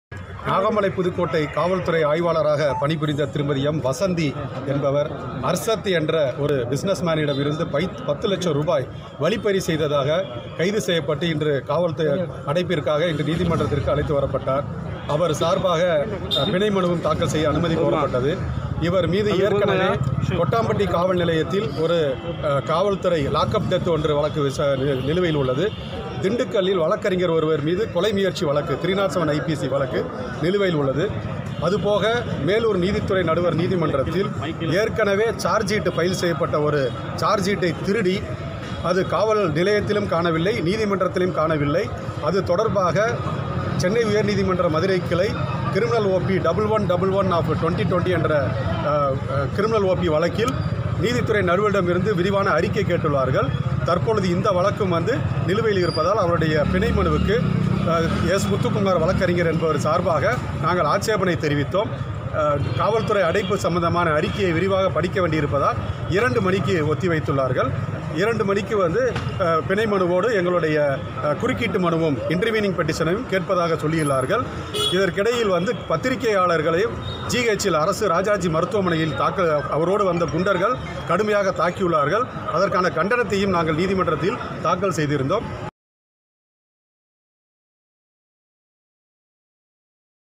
Fraud case | பெண் காவல் ஆய்வாளர் வசந்தி சிறையில் அடைப்பு | 10 Lack fraud case | ETV Bharat Tamilnadu
இளைஞரிடம் 10 லட்சம் ரூபாய் மிரட்டிப் பறித்த வழக்கில் நாகமலை புதுக்கோட்டை காவல் ஆய்வாளர் வசந்தியை செப்டம்பர் 9 வரை நீதிமன்றக் காவலில் வைக்க மதுரை மாவட்ட கூடுதல் மகளிர் நீதிமன்றம் உத்தரவிட்டுள்ளது.